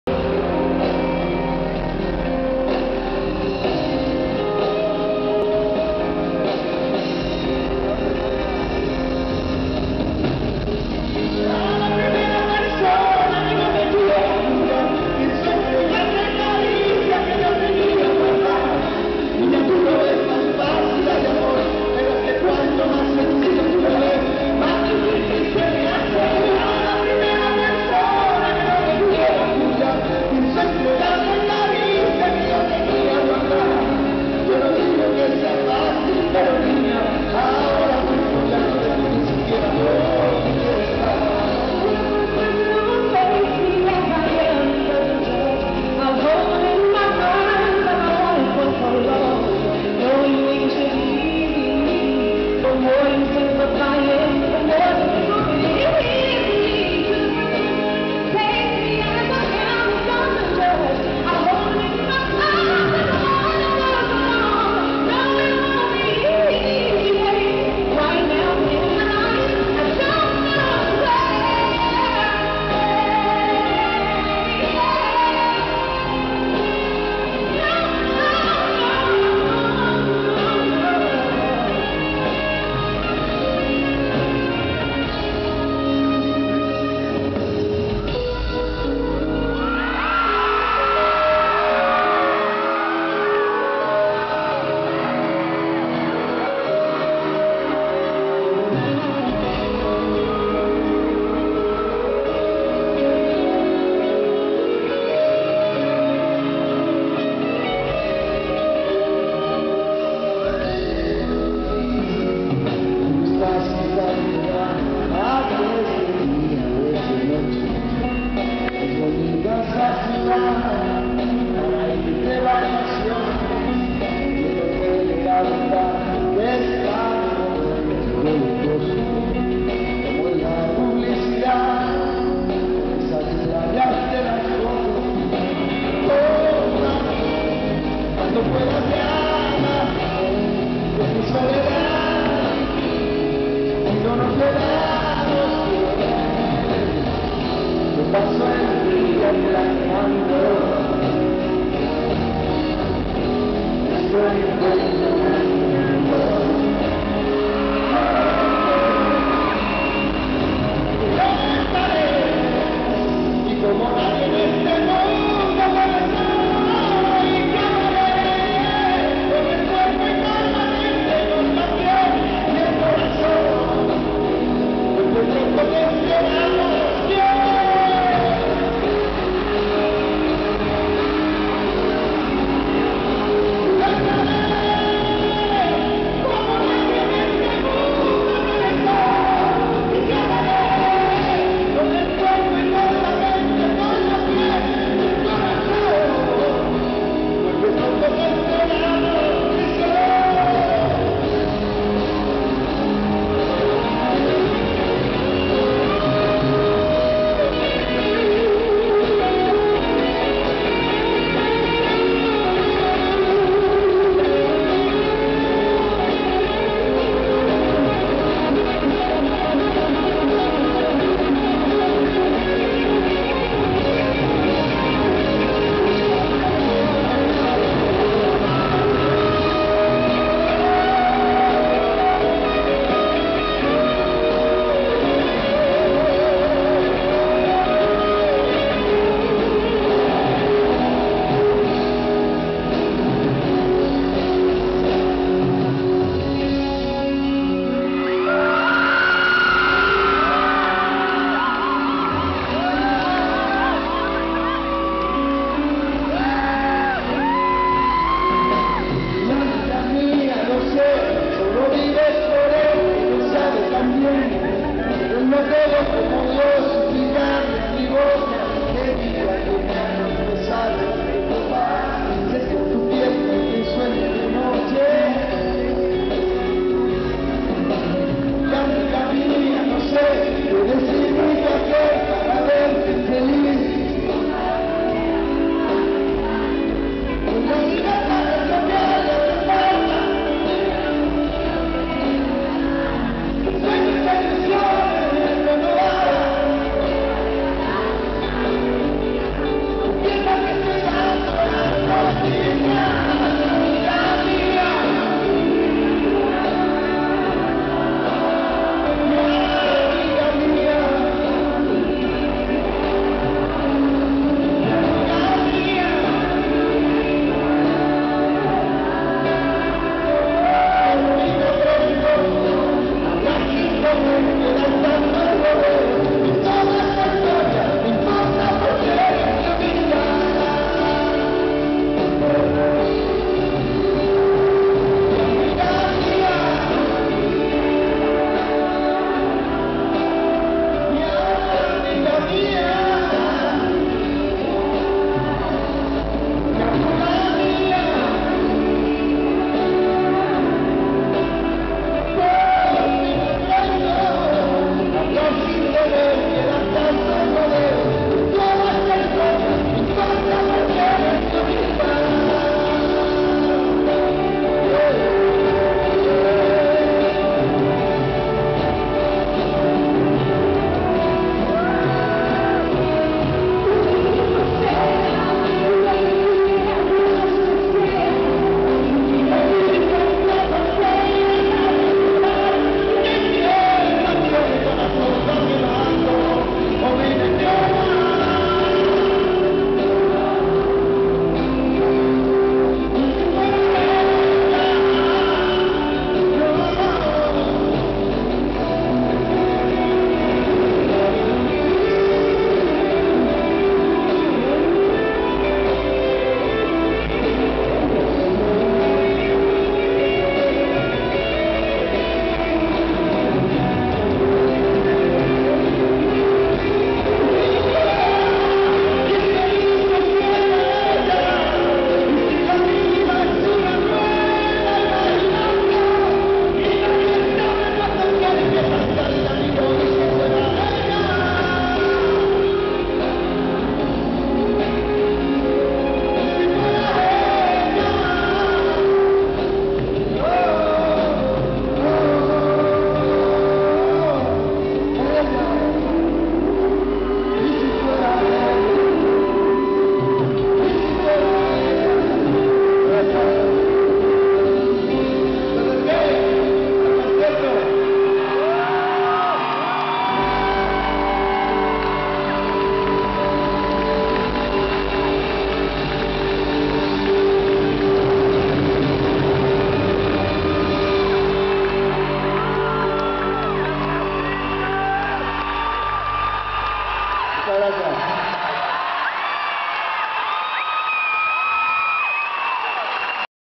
Alejandro Sanz - Popurri (finale) (HP Pavilion)
Alejandro Sanz concert on 07-18-10 at the HP Pavilion in San Jose, CA